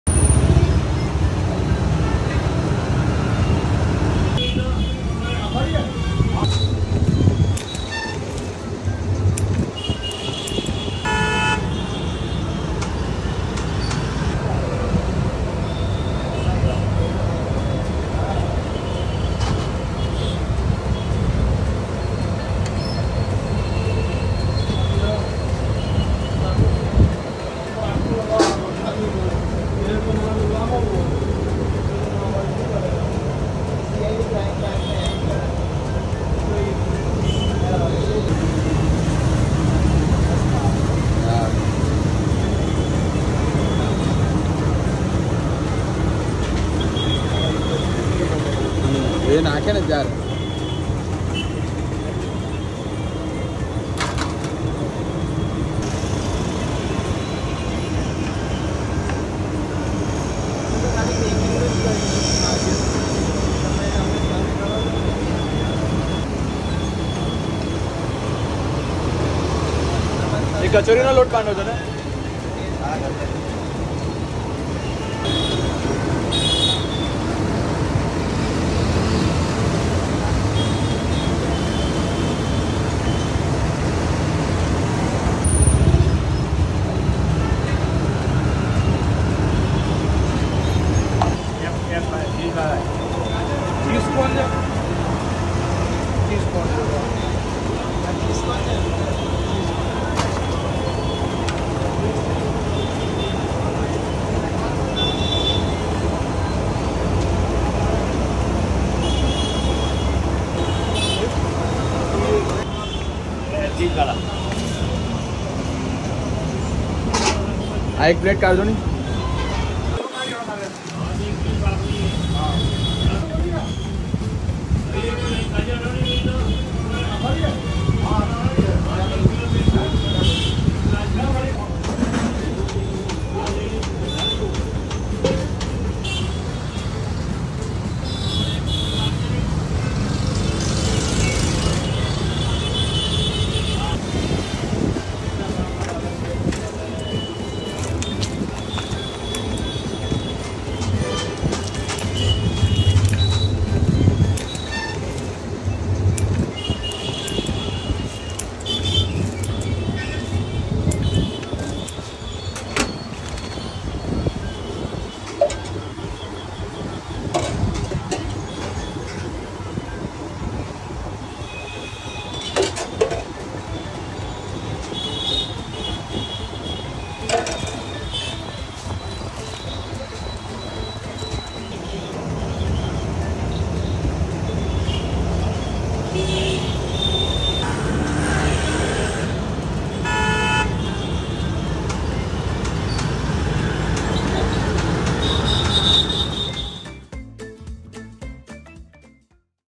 Maninagar Famous Cheese Samosa and Pyaaz Kachori 😍 | Nagar Farsan Corner | Indian Street Food
Address: Shop no .3, white house Nr. Jagariya bridge,l g corner, Maninagar, Ahmedabad, Gujarat 380008